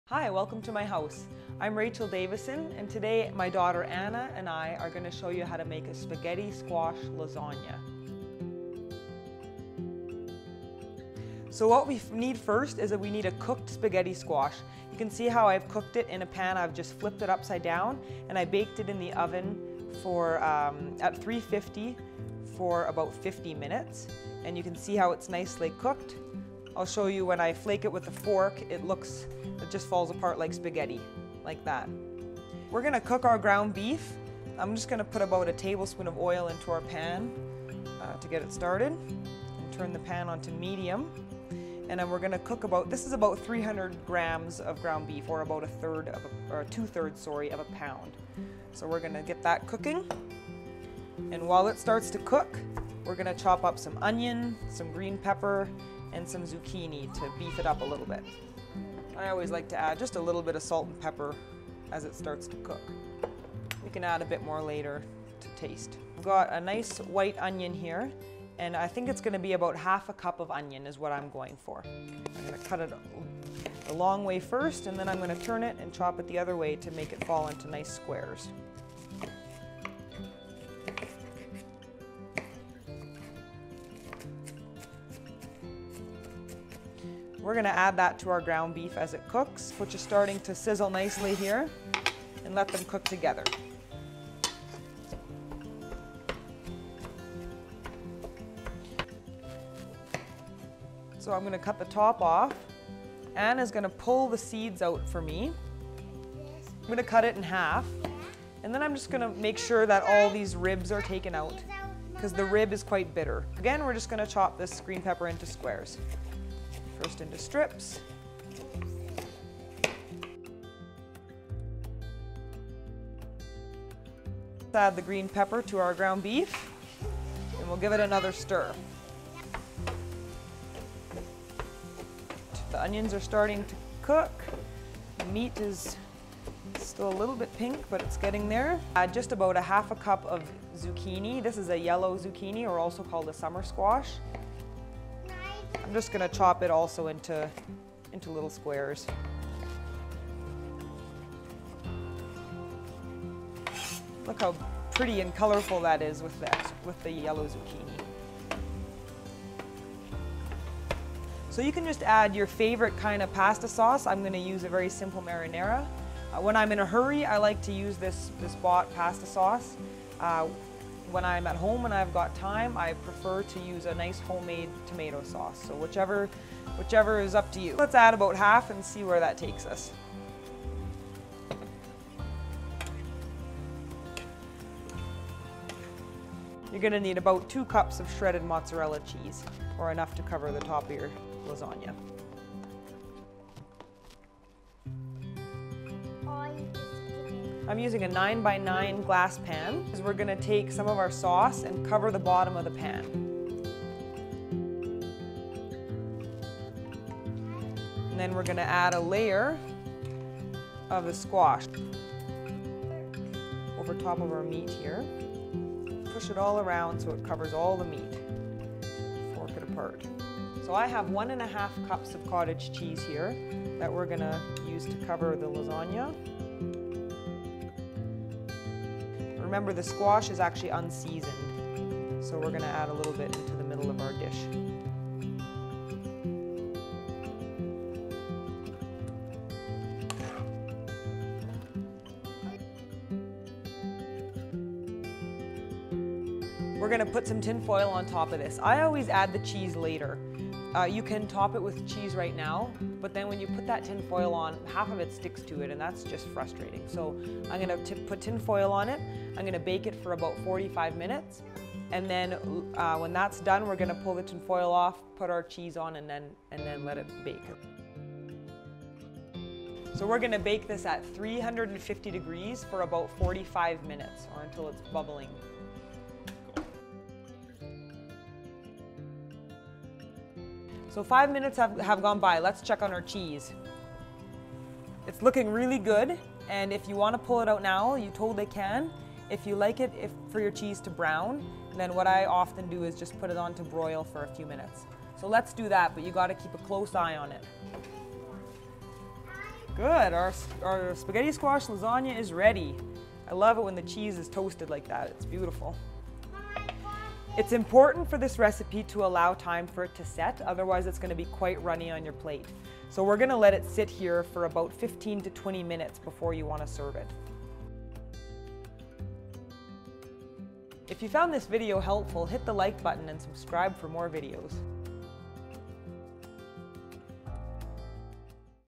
Family Recipes -- Spaghetti Squash Lasagna
Follow along with Rachel and Anna Davison as they make this delicious seasonal delight: "Spaghetti Squash Lasagna"!